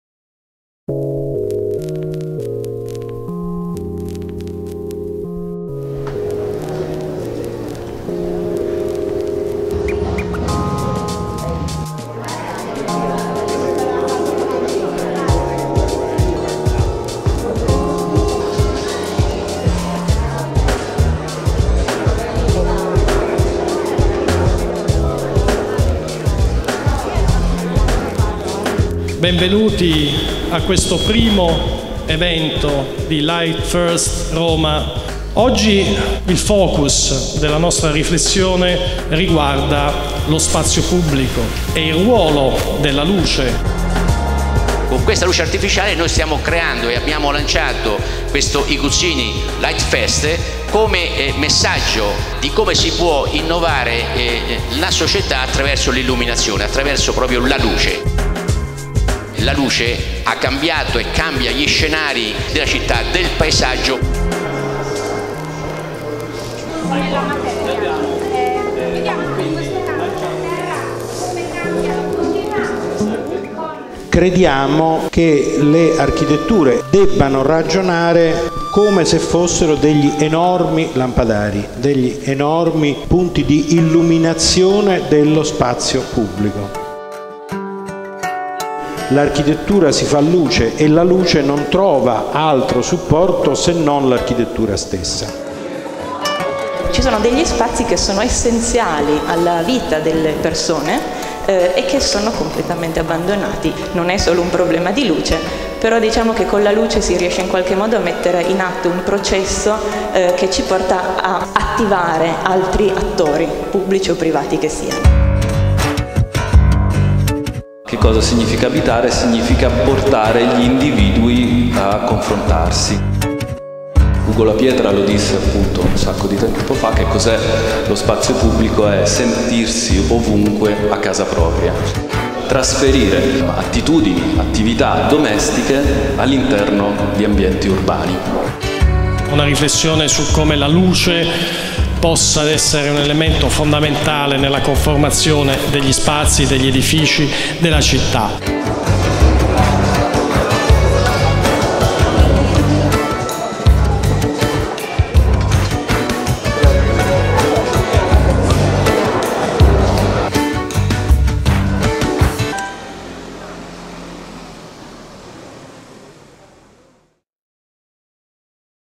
OPEN DAY - Roma Officine Farneto - iGuzzini - FR
Un lieu exceptionnel d’un point de vue architectural. Une opportunité extraordinaire de rencontre pour dialoguer avec des professionnels de la lumière. Une occasion unique de voir et manipuler toute la gamme de nos nouveaux produits.